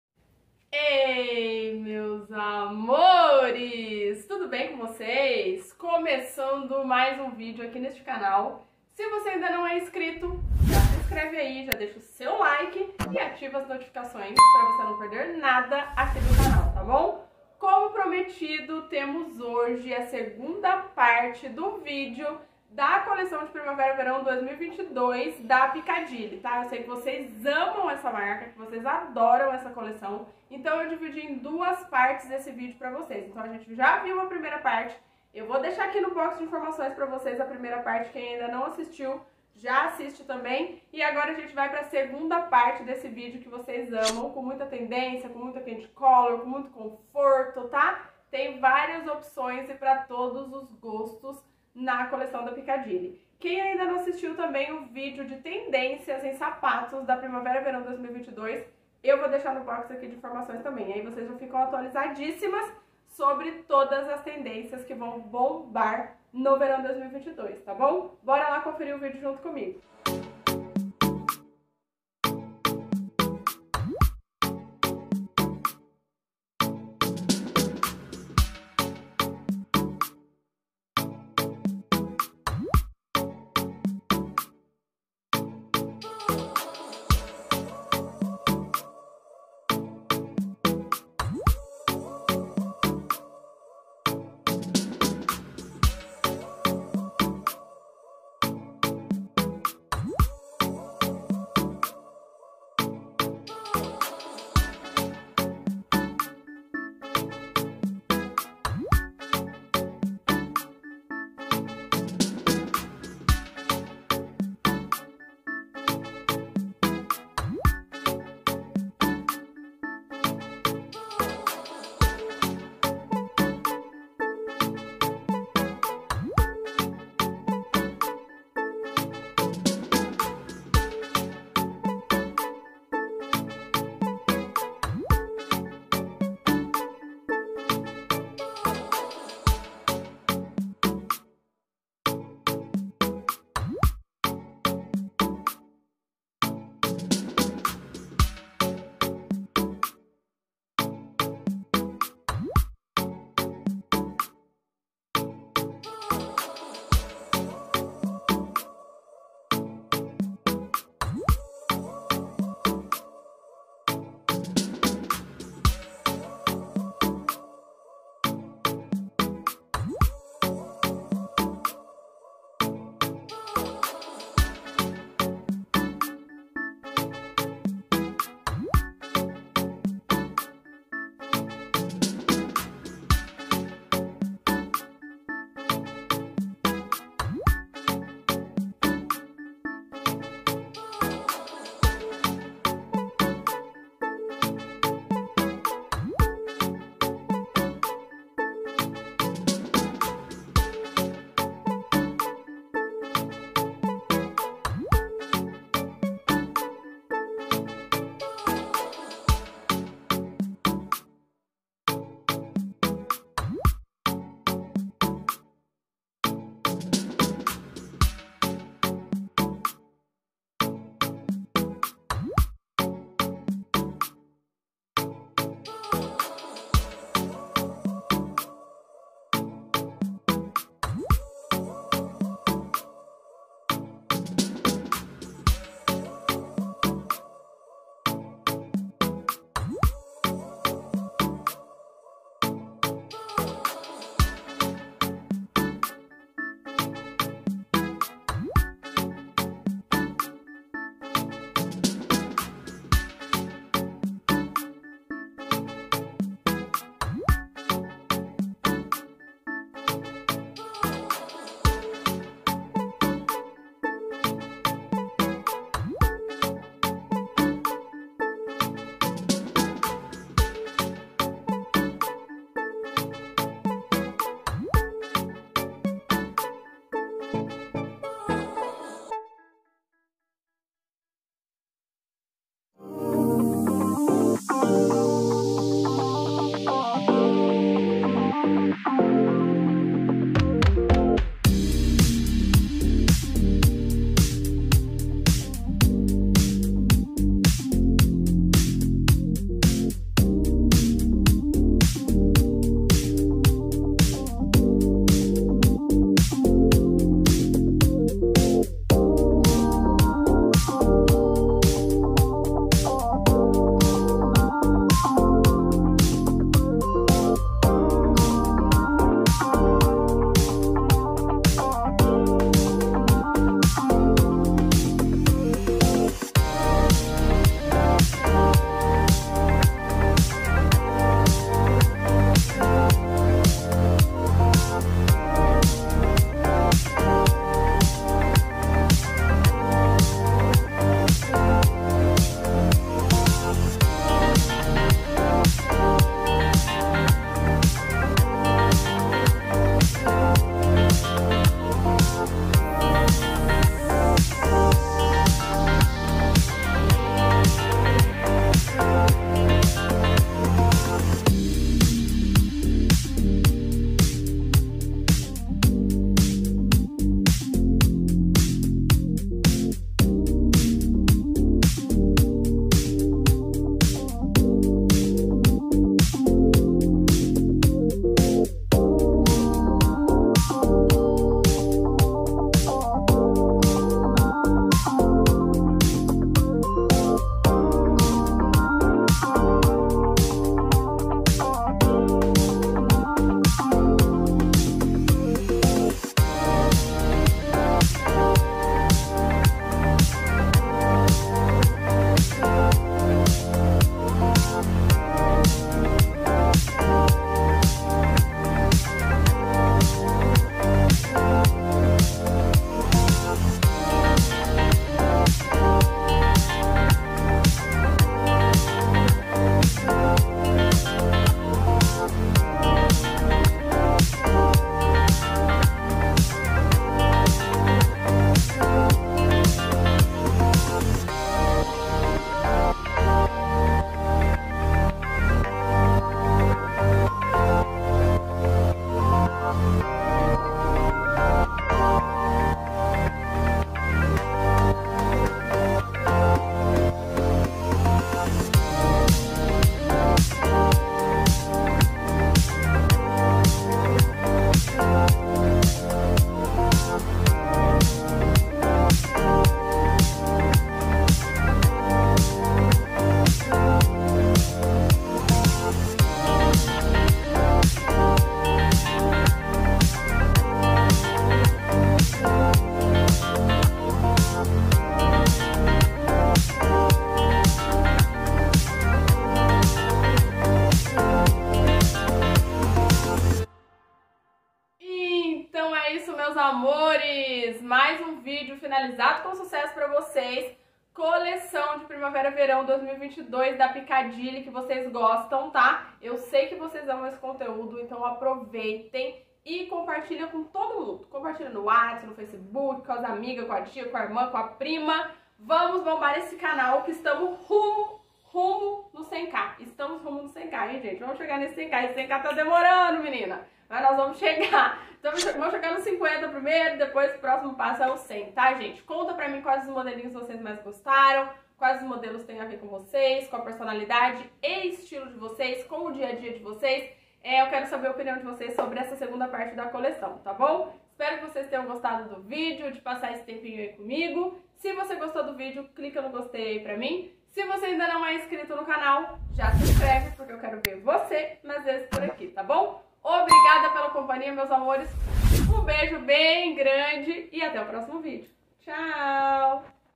PICCADILLY COLEÇÃO VERÃO 2022 CALÇADOS LANÇAMENTOS PICCADILLY E TENDÊNCIAS | Caren Sales
ALGUNS CALÇADOS QUE SELECIONEI PRA VOCÊ:

Tenis Slip On New Blue Em Elastano Com Costura Enfeite 33

Tênis Sneaker Bicolor Branco De Couro Com Fecho-Flex 37

Tênis Plataforma Bicolor Lavanda De Couro Com Tela 33

Mule Preto Bico Quadrado De Couro Matelassê Pelica 33

Tênis Slip On Quartzo Em Elastano Com Monograma 33

Mule Light Blue Bico Quadrado De Couro Croco Com Fivela 33